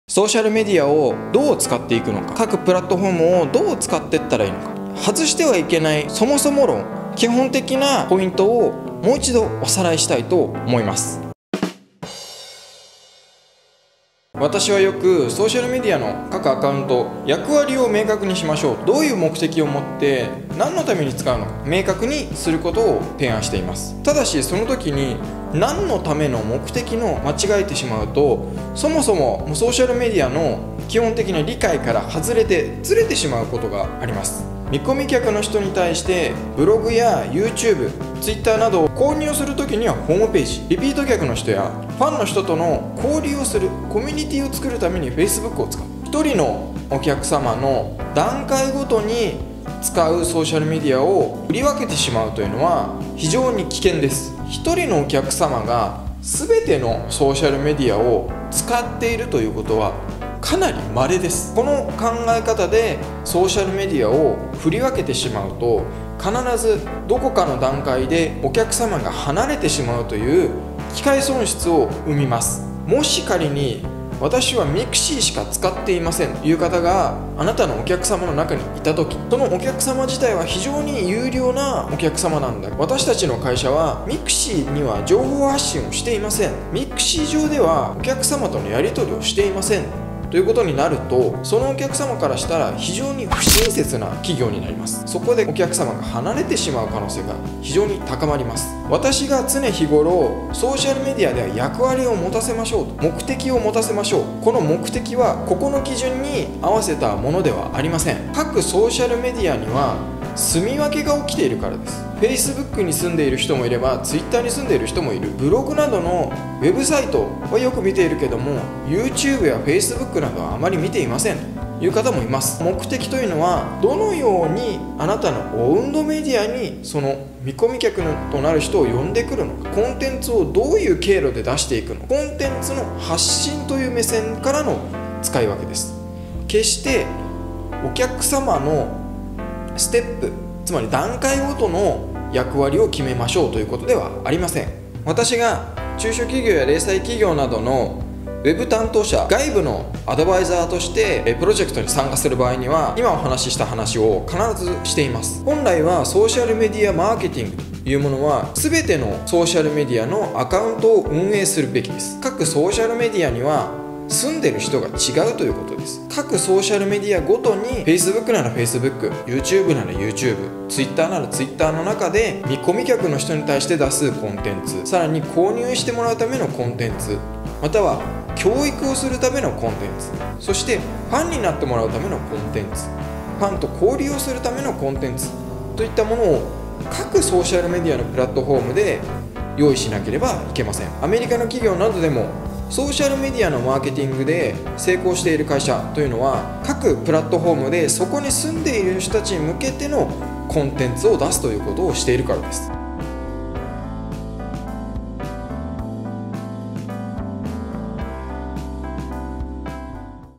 ビジネスでソーシャルメディア運営に取り組むときの落とし穴
ソーシャルメディアの各プラットフォームを1人のお客様のステップごとに割り振るのは危険！そうではなくてコンテンツ目線で役割を決めよう！